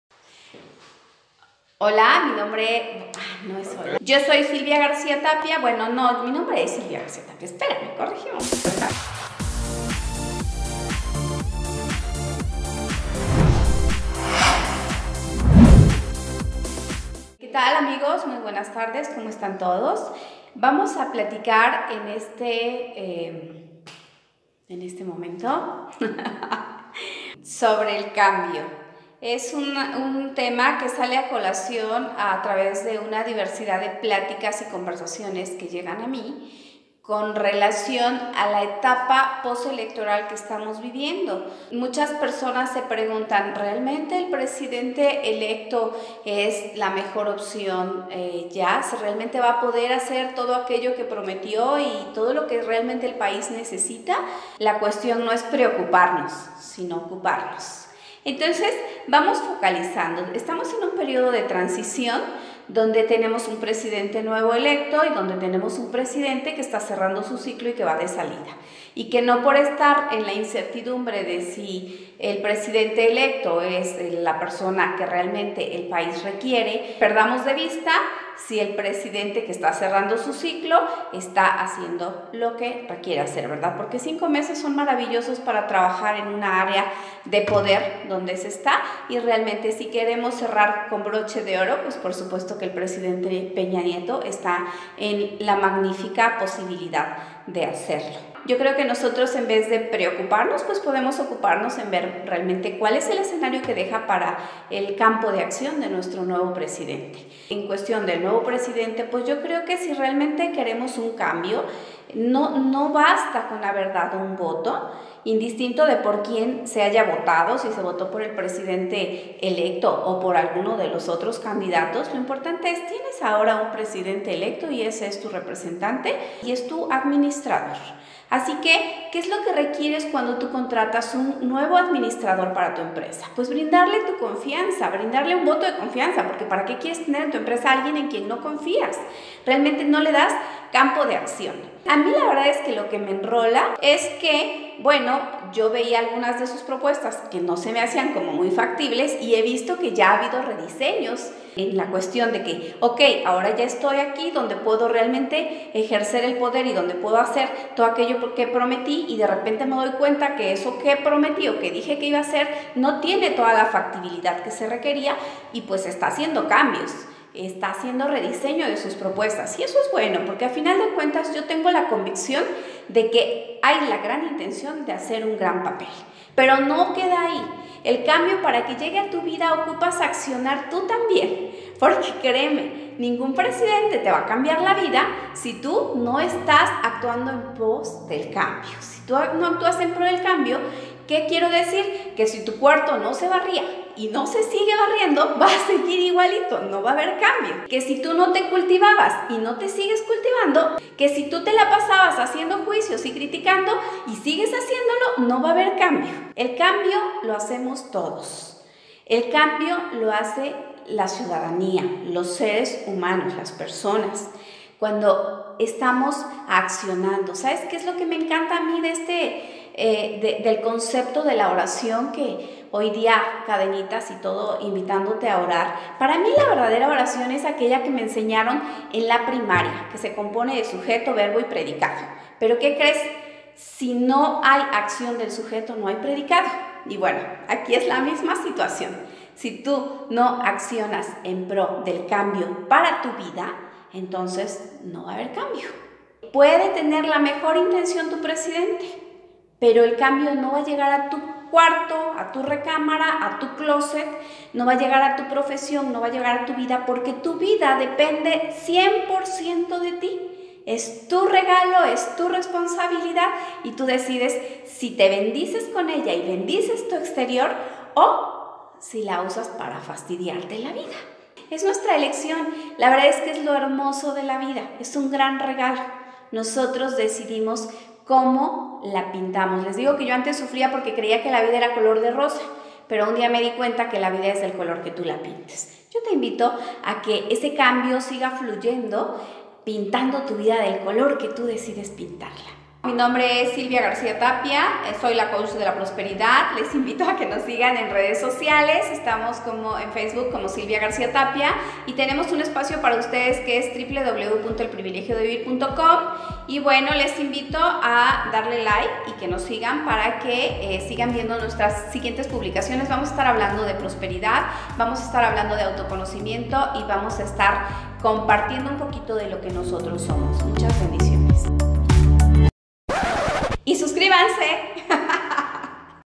CONFIAR EN AMLO | Elecciones México 2018 | Silvia García Tapia
Andrés Manuel Lopez Obrador, fue el ganador de las Elecciones presidenciales en Mexico este 2018, lo cual dio mucho de qué hablar; infinidad de personas se sentían divididas y con incertidumbre.

Hoy hablo de como tratar estos conflictos y lograr enfocar tu energía para cumplir tus metas, tú tienes el poder para cambiar estas cosas y yo te diré como.

¡Nuevo video todo los martes!